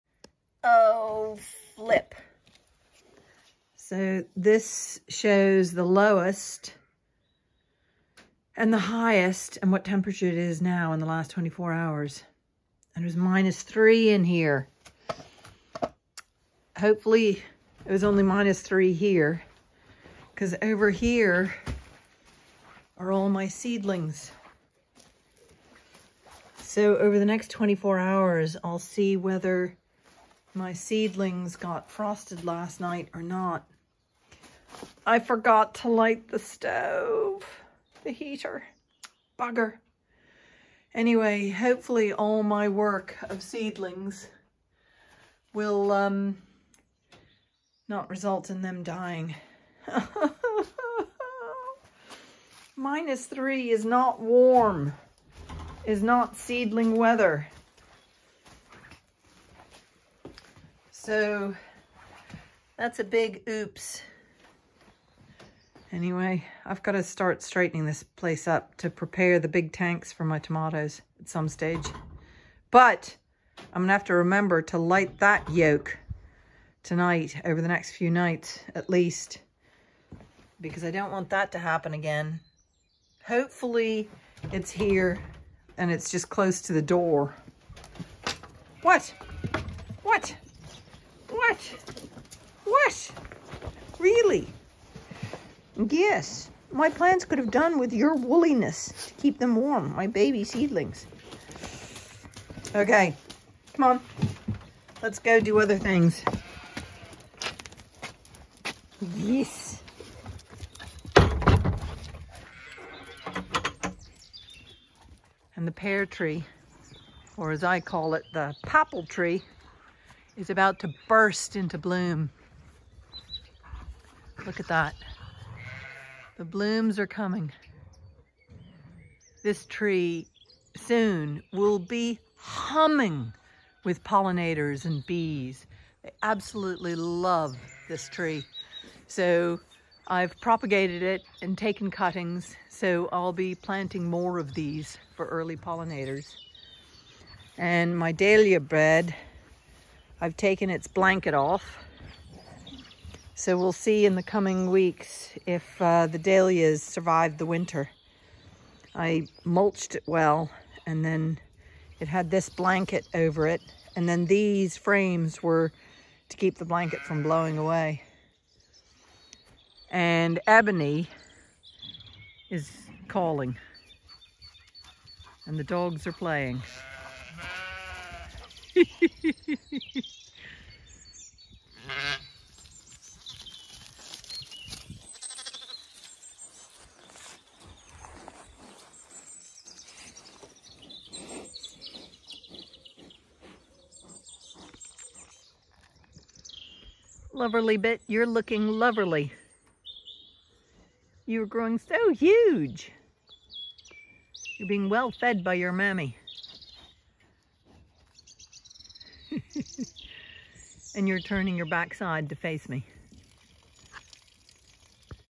Oh filbidygibit & feckity feck, I forgot to light the heater last night
Hopefully my newly transplanted seedlings will not have been shock frosted!!! I shall see in a few day if disaster has struck. Meanwhile LuverlyBit is growing stronger and bigger every day.